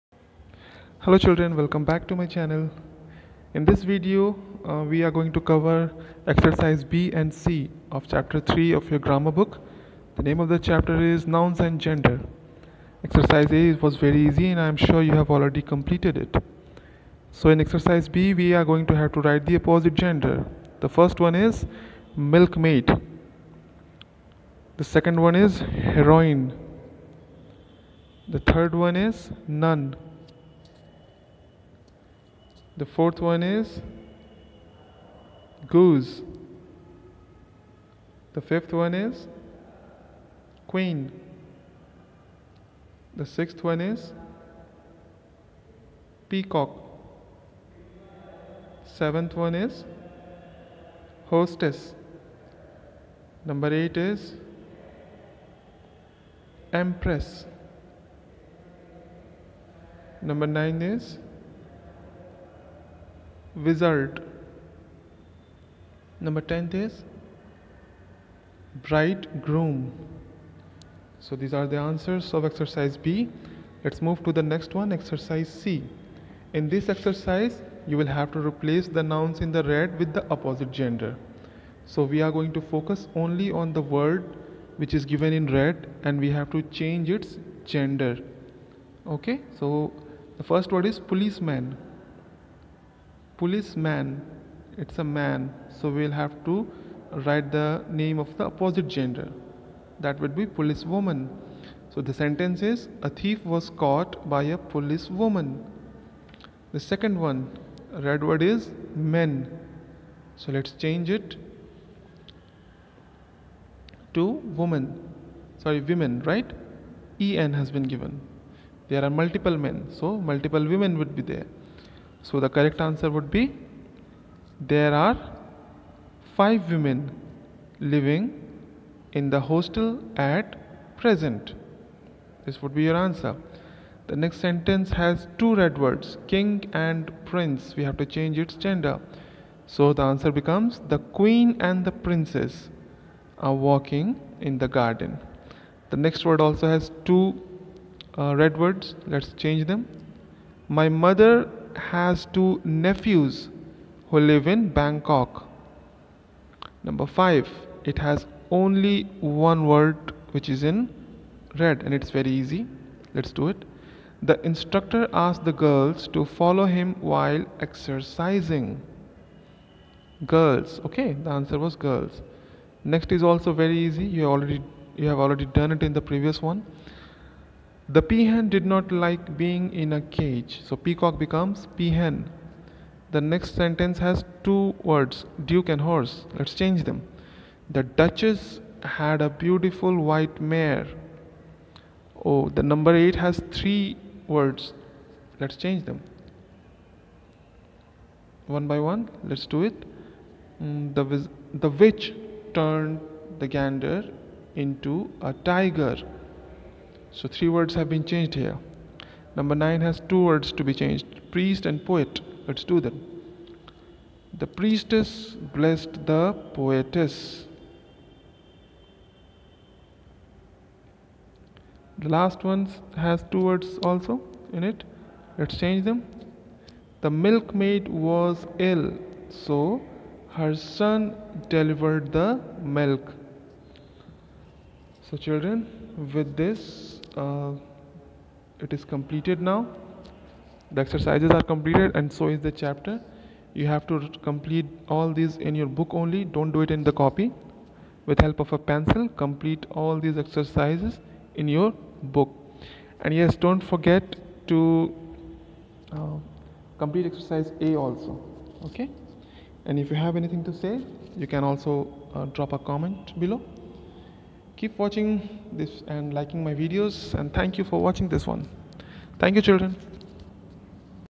Class V, Noun- Gender, excercise B and C solution
This video contains the solution of exercise B and C of chapter: nouns gender, class V.